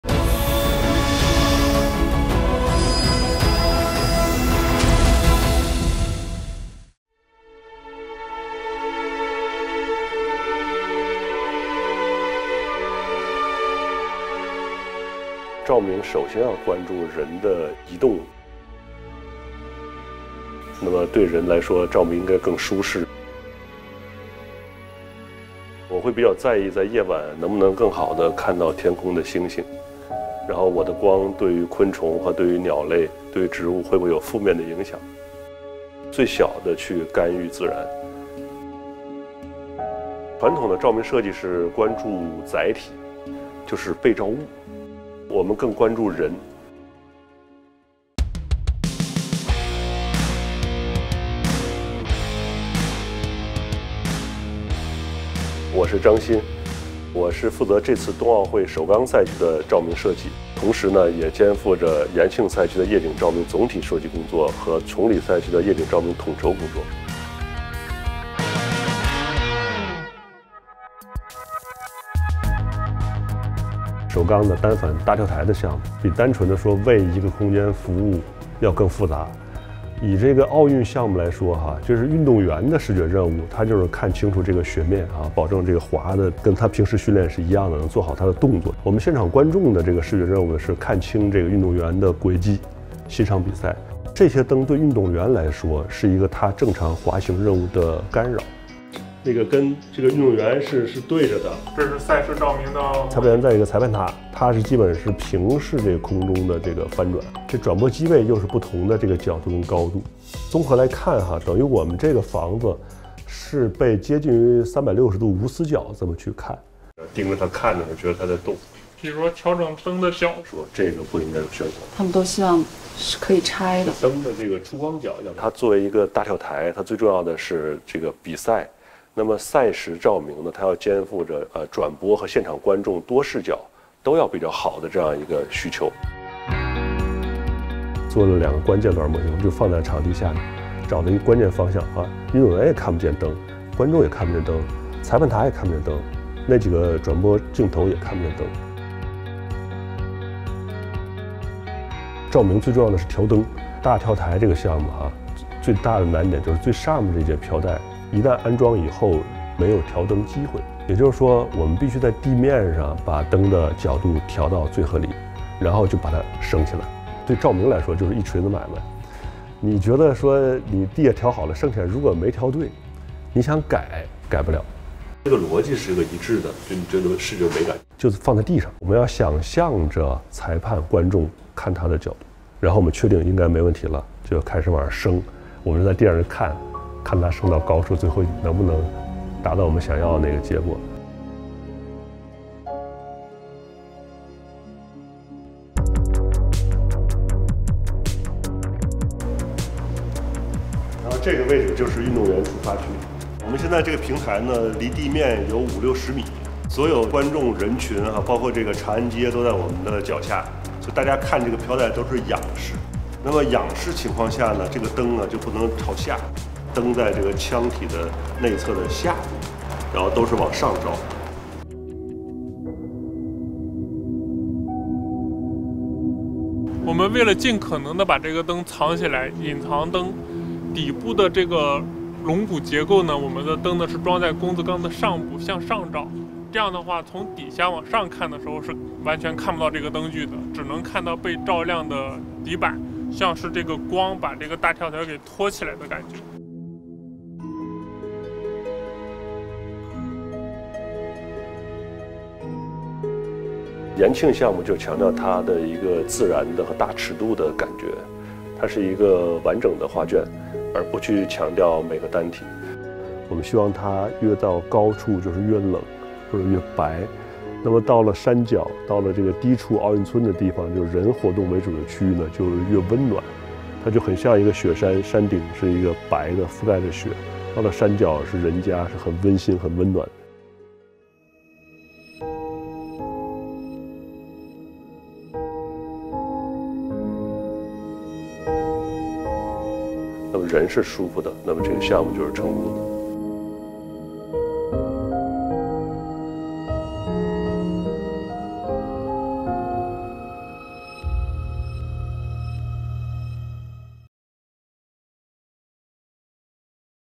Humans of Beijing 2022: Zhang Xin, light designer of Winter Olympics
Light and shadow are indispensable elements of aesthetics. The Olympic flame has encouraged people to promote sports, and thousands of lights will illuminate all competition zones. Let's see how the light designer Zhang Xin plans to illuminate the venues.